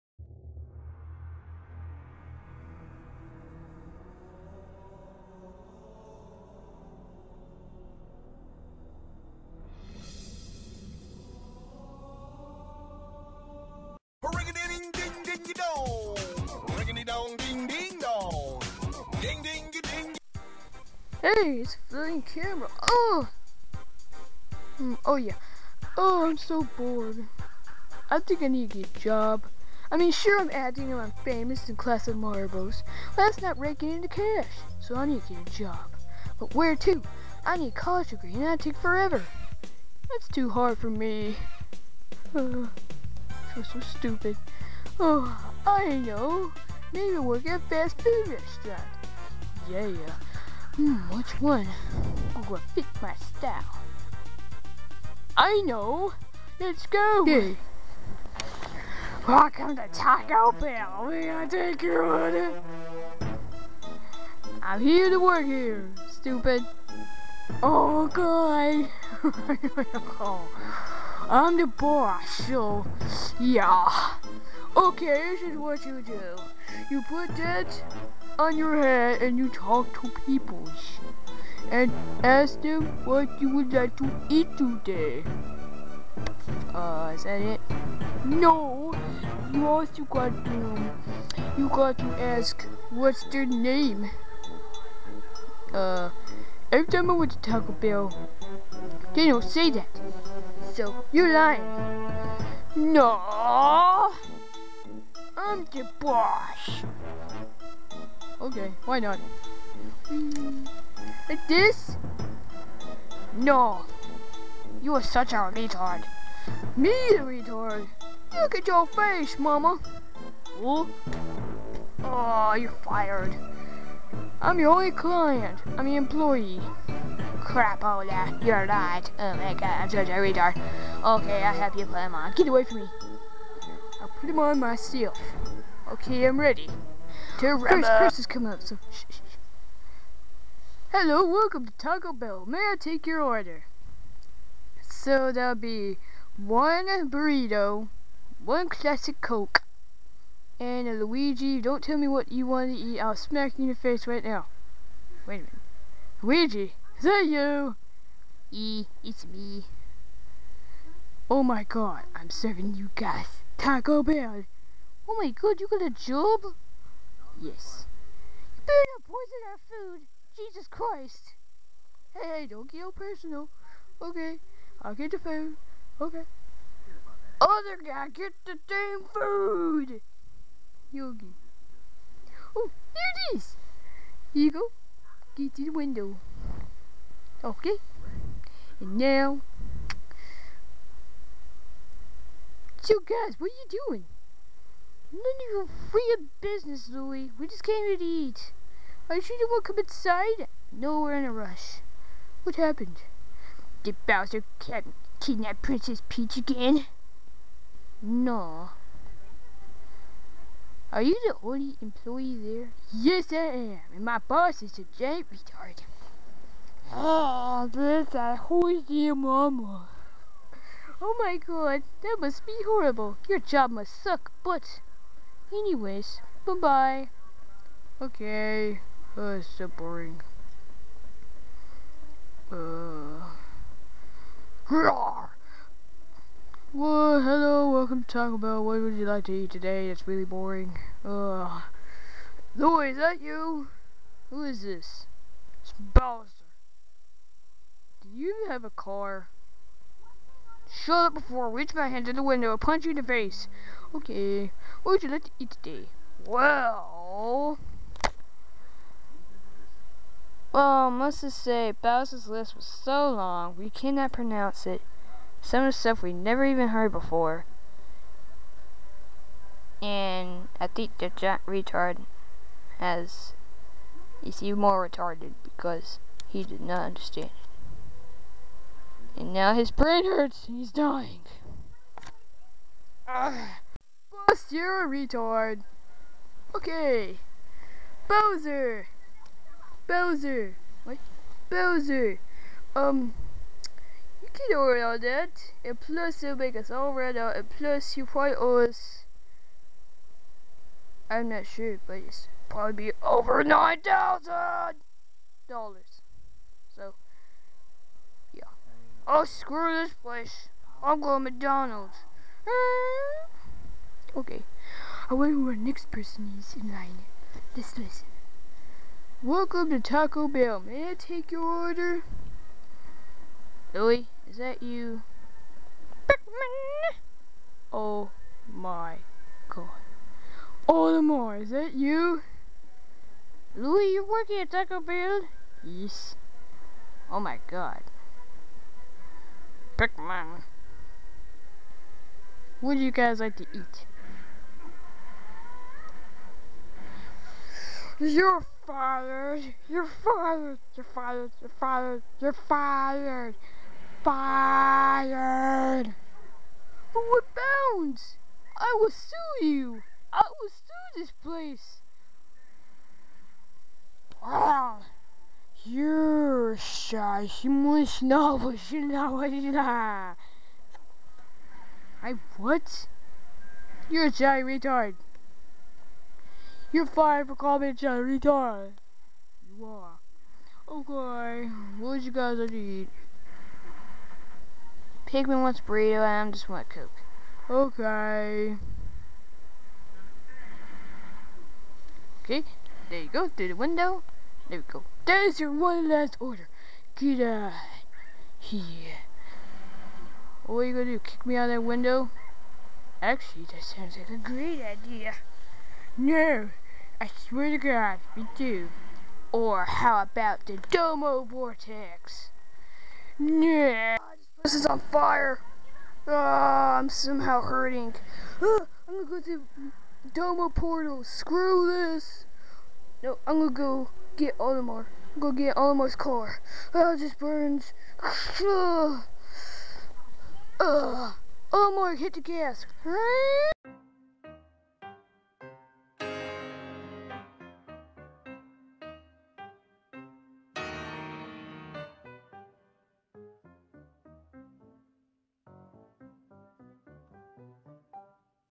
CMB Short: Louie IN: Job At Taco Bell (Pilot Short)
Basically in a nutshell, Louie get's a job at taco bell and his boss is a "retard". Everything turns into...